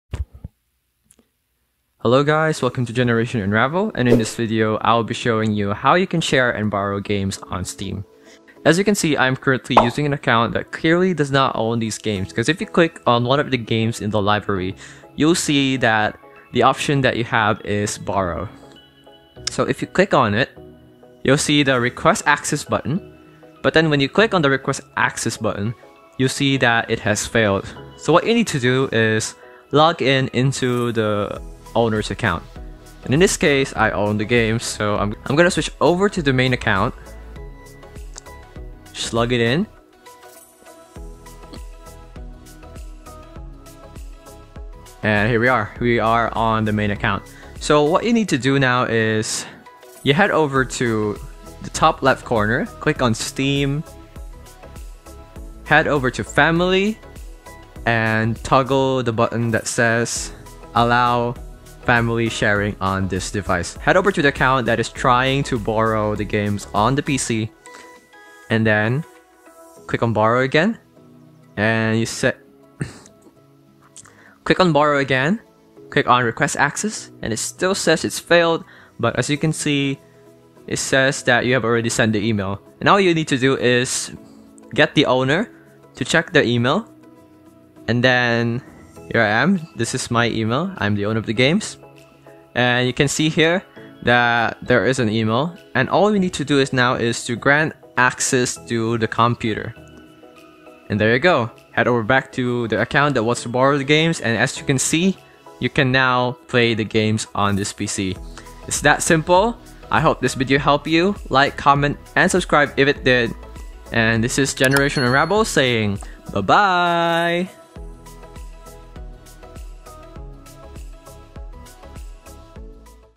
How To Share and Borrow Games On Steam
Do you want to share and borrow some games with your friends? Watch the video and this might be an option for you.